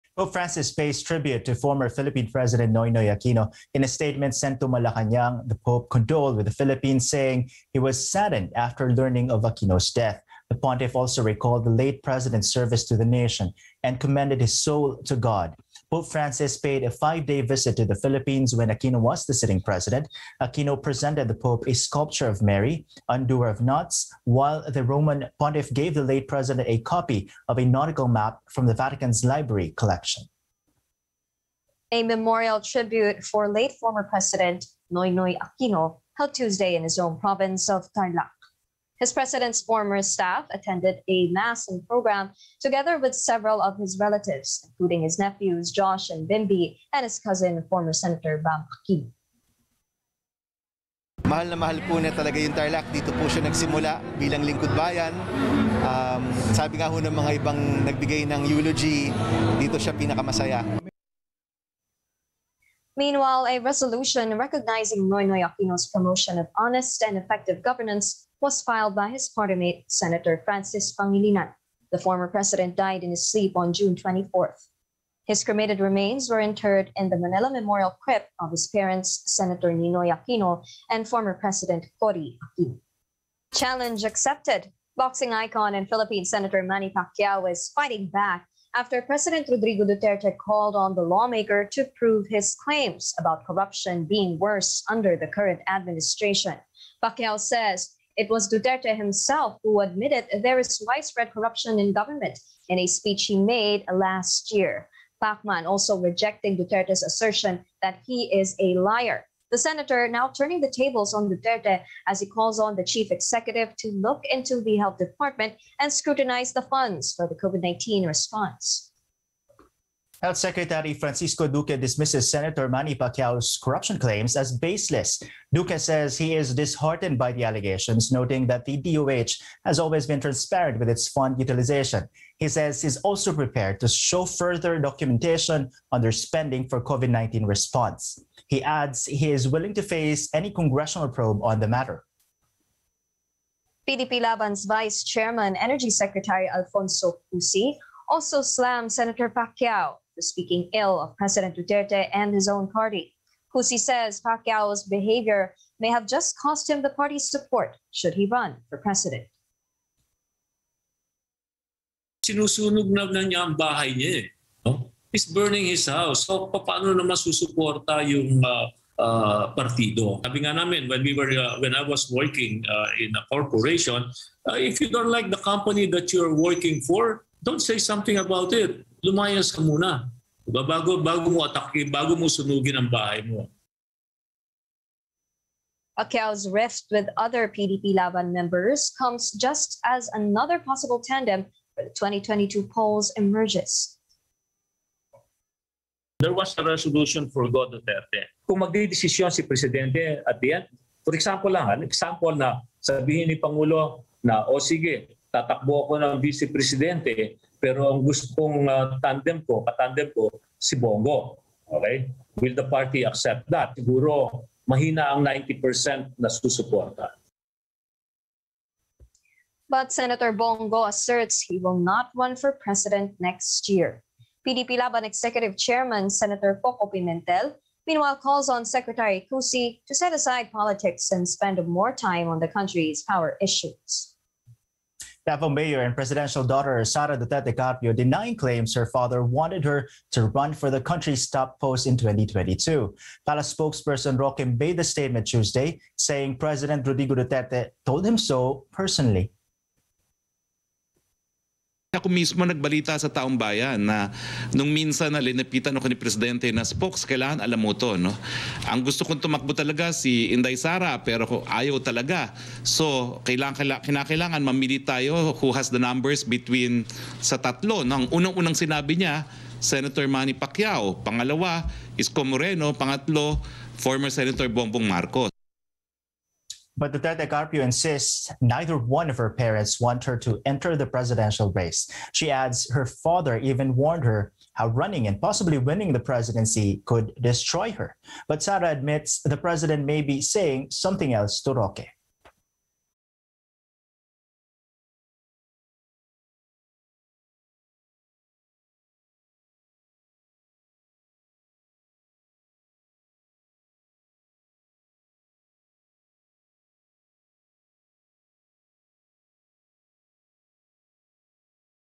Pope Francis saddened over Noynoy Aquino's death | ANC
Rundown: Pope Francis pays tribute to former Philippine President Noynoy Aquino. The pontiff recalled the late president's service to the nation and commended his soul to God.